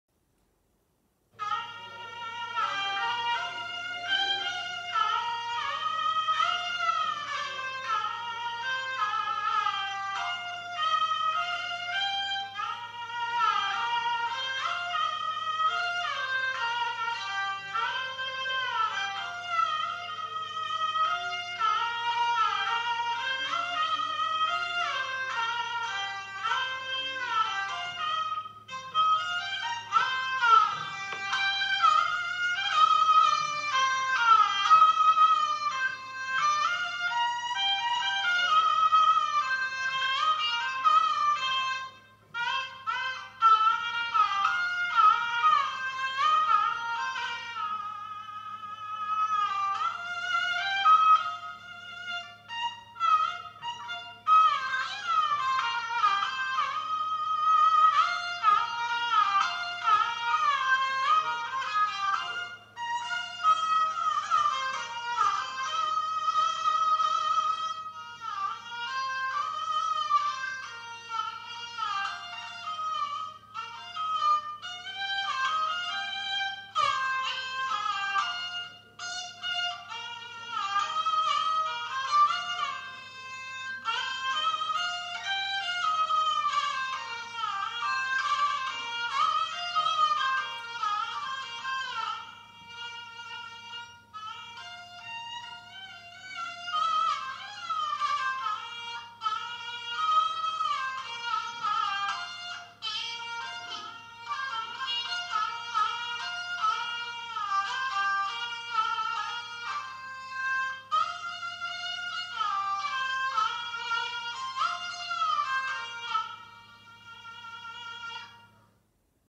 Erhu Solo (Vacuum tube hifi)
Erhu class at Honan Theatrical School, recorded by Guy and Candie Carawan, March 1976
It sounds like a 京胡, high-pitched erhu designed to accompany the Peking opera.